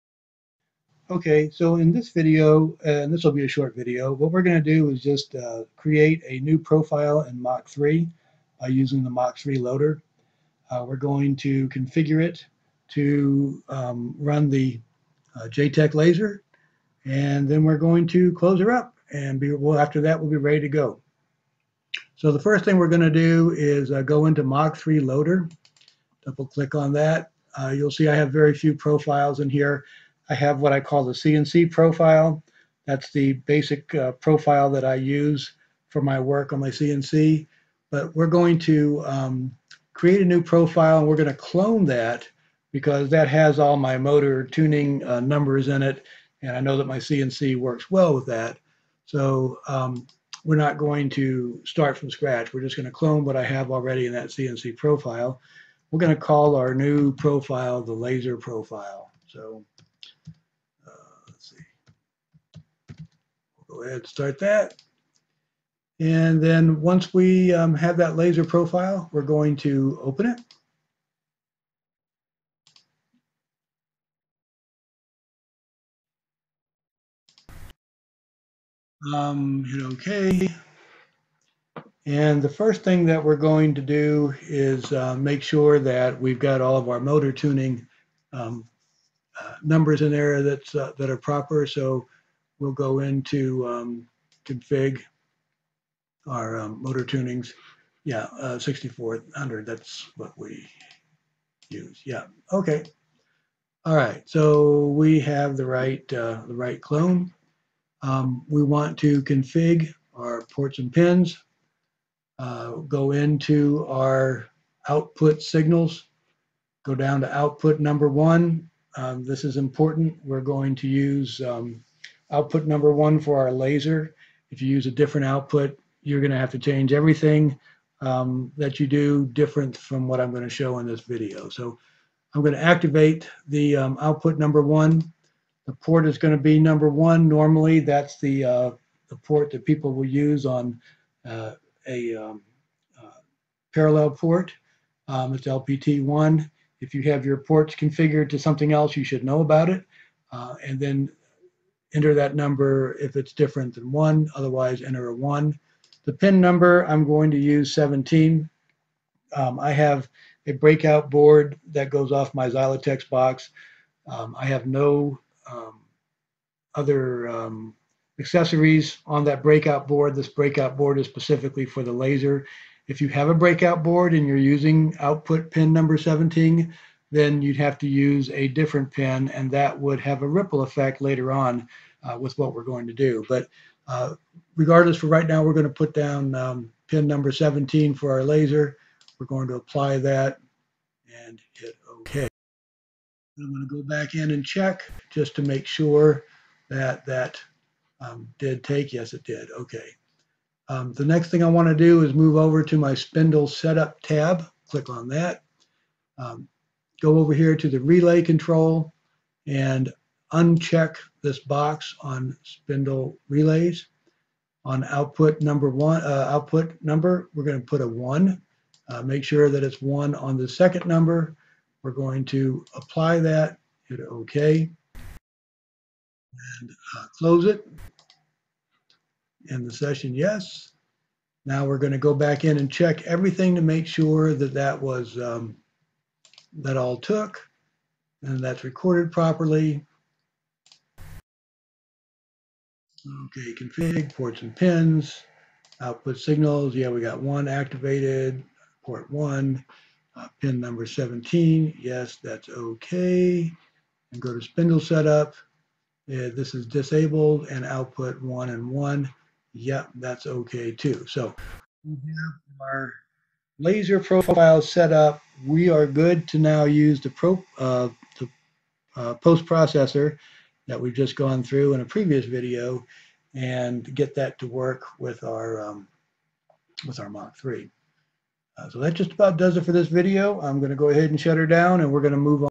0002 Mach 3 Laser Profile Setup for JTech Laser and VCarve Pro
This video demonstrates how to set up Mach 3 for the JTech Laser and the modified Post Processor